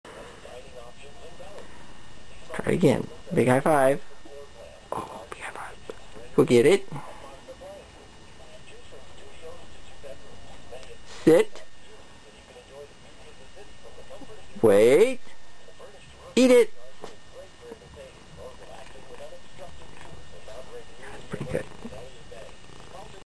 Fluffy Wait Trick
Took only a dozen or so times to teach this smart girl to wait, though it didn't always work.  She almost instantly knew how to do high fives and shake.